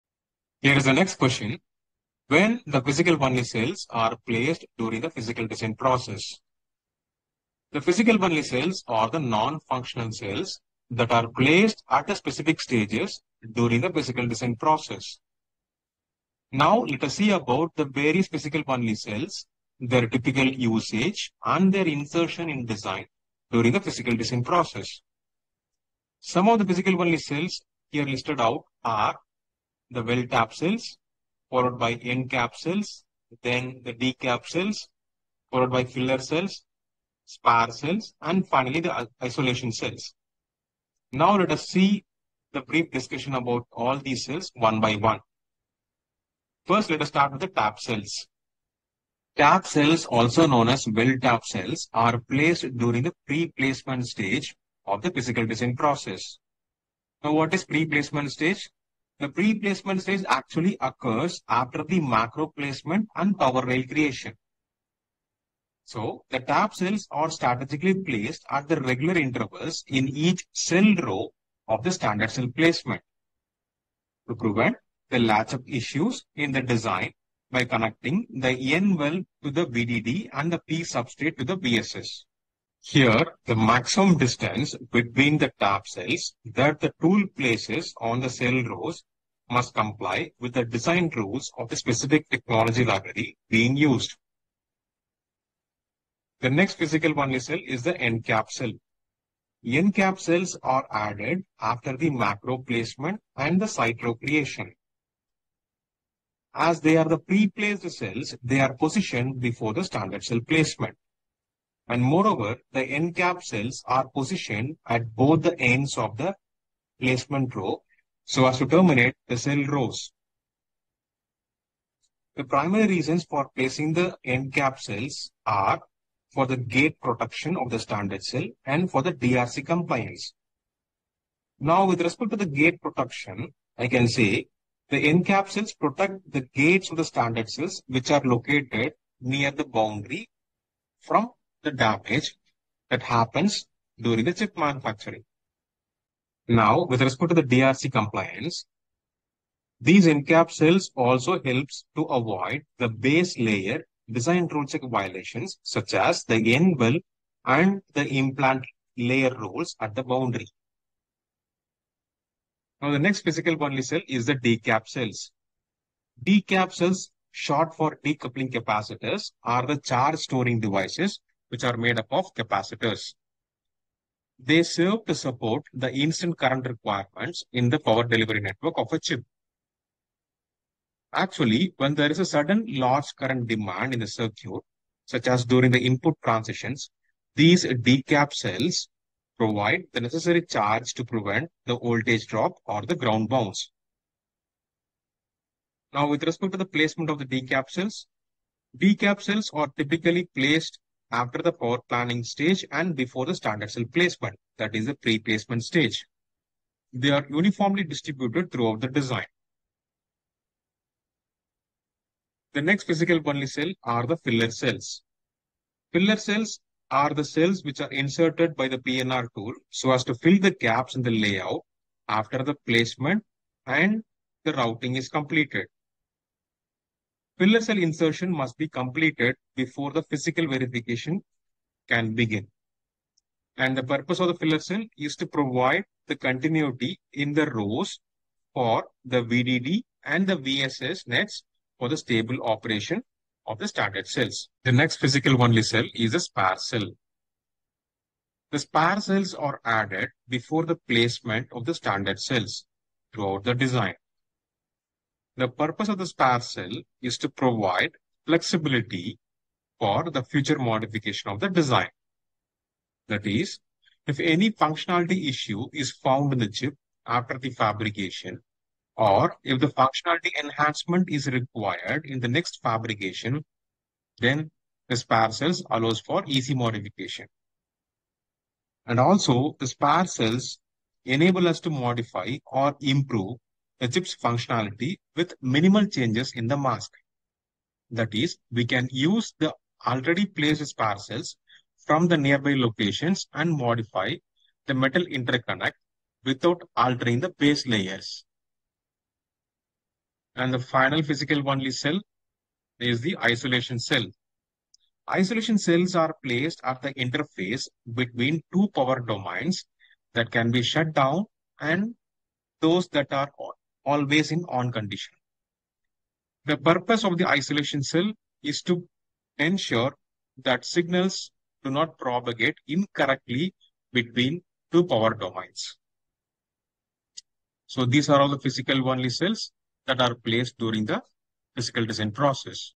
L2S1_Interview- Placement of Physical Only Cells
Physical-only Cell placement at which stage and where in the VLSI design is very important from the question point of view. Why we are placing a physical-only cell at a particular stage - Interview always need to know.
In this video, we have summarized this.

This is the part of Interview series